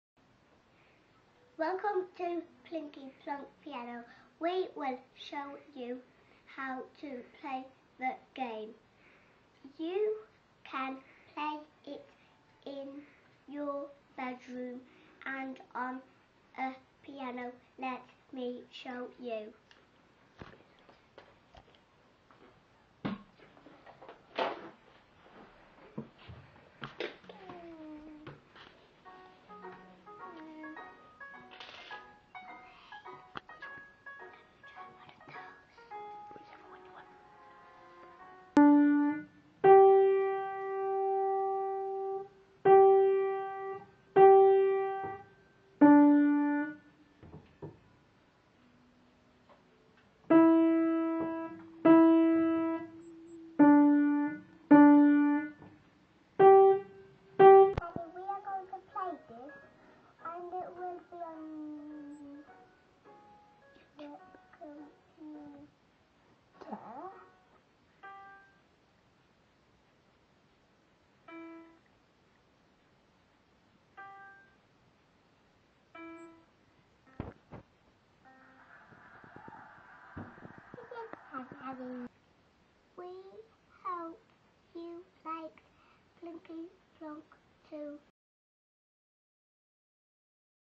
Plinky Plonk Piano Demo
Here is my 5 year old explaining how to use Plinky-Plonk Piano.